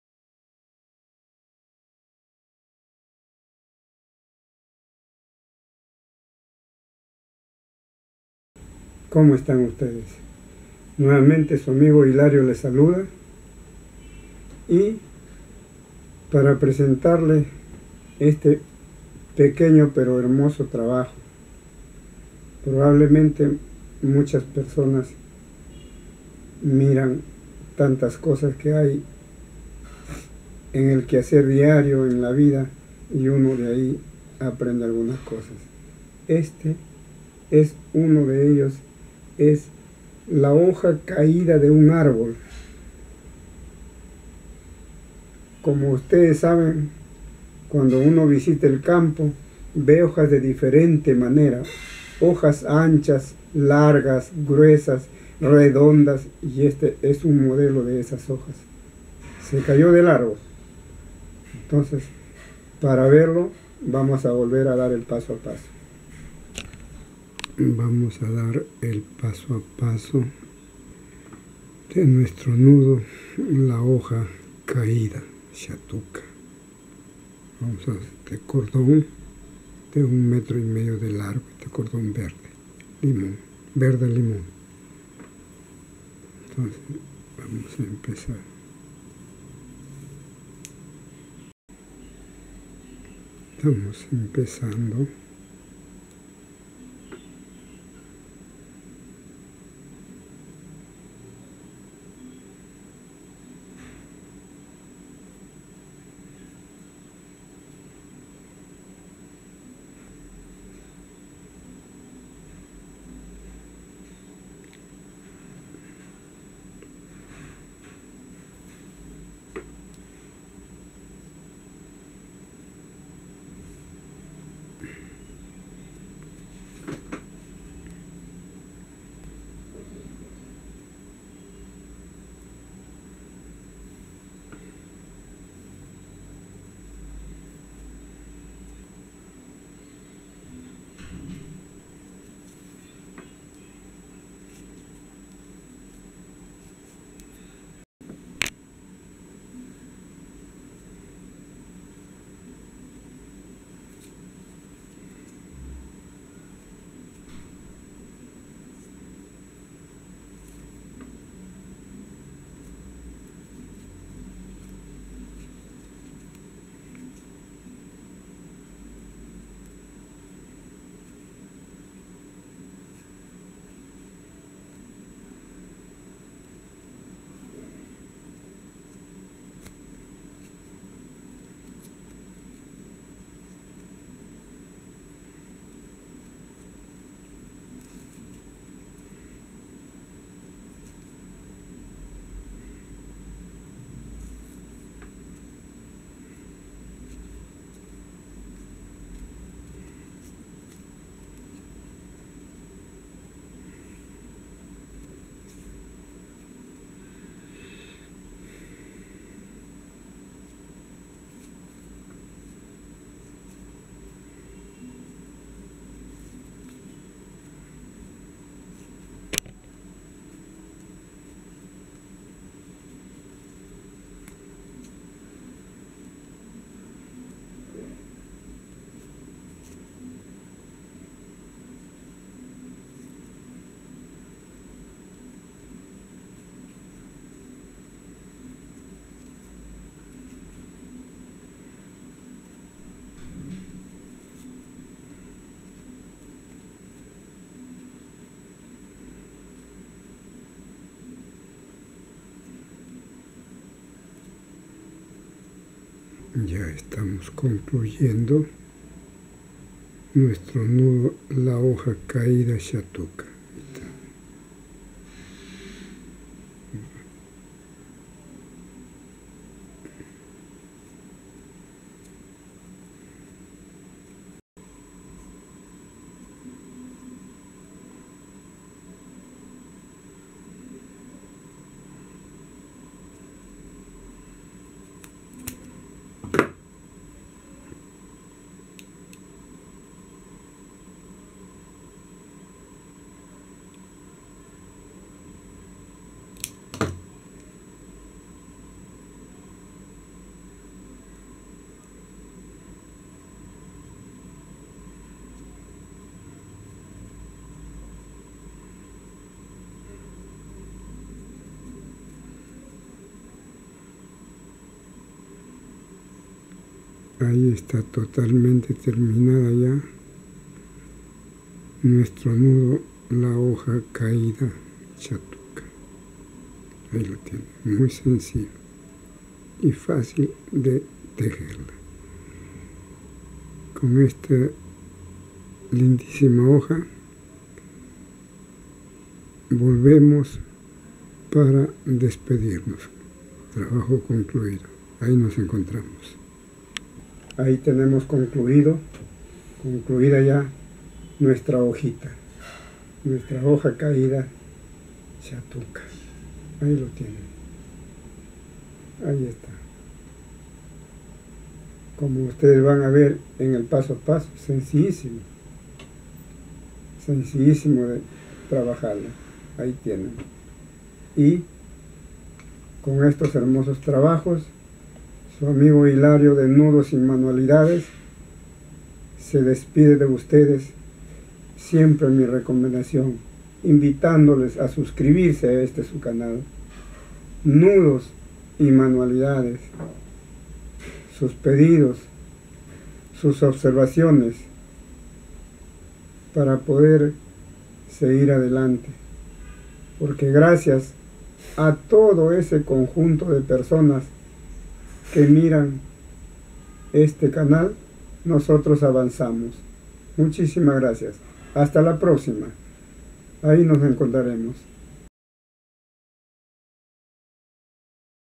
NUDO HOJA CAIDA - SHATUKA
Presentamos este este nuevo proyecto de nudo hoja caida, un lindo trabajo hecho a mano, les invito verlo y realizarlo. Saludos y Buen fin de semana.